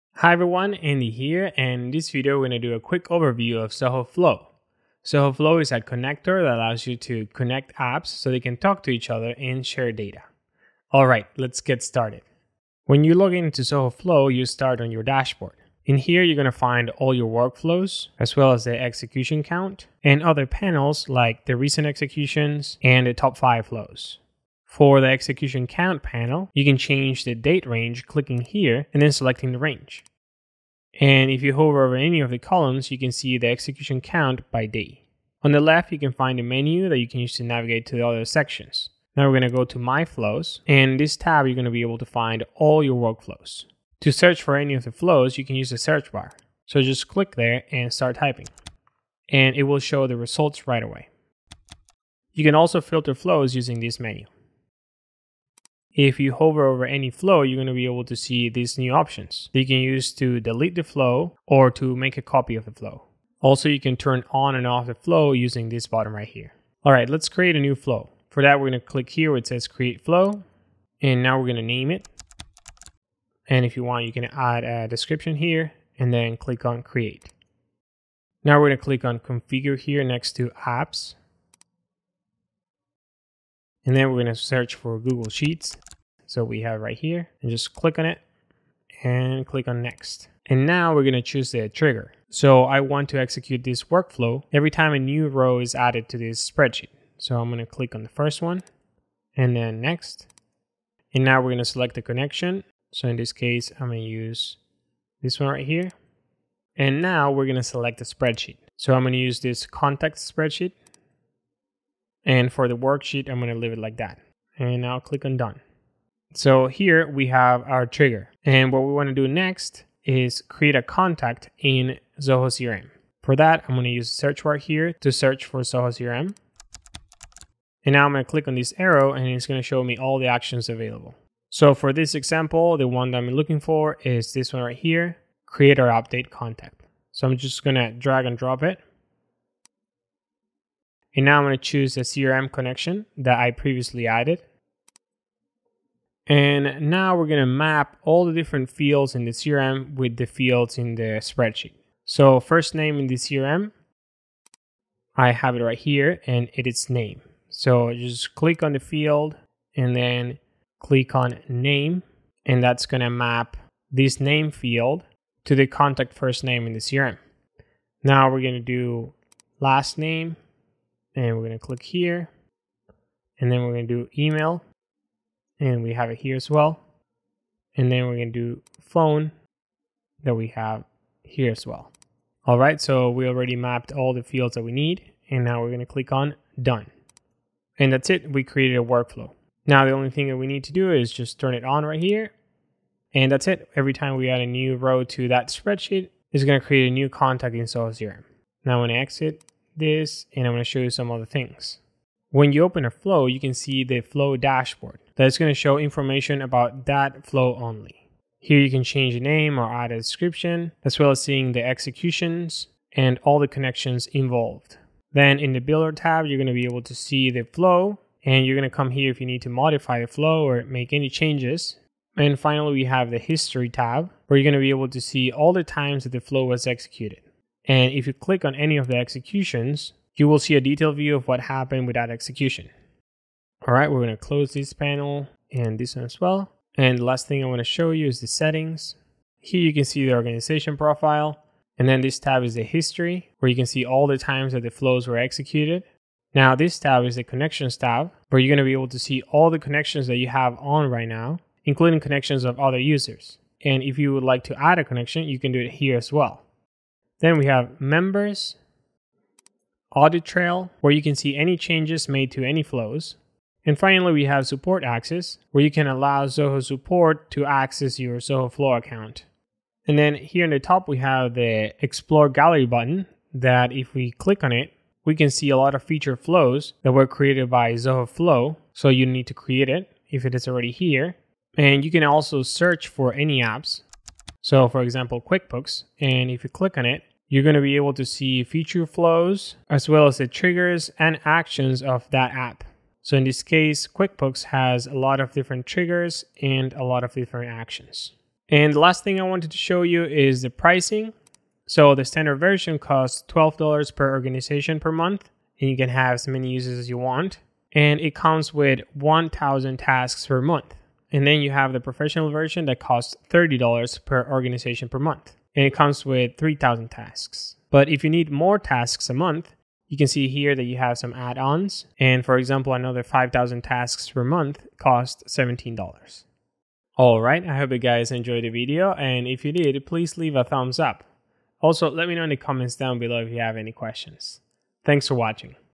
Zoho Flow Overview: Connect apps together!
- Create a FREE Zoho One account (Zoho Flow included) using this

In this video we are going to go over all the features of Zoho Flow. With this app you will be able to create automations, connecting different apps so they can to to each other. You can create workflows that executes when an event happens, called a trigger, and then you can set up the actions that will be performed. I will also show you how to create a basic flow.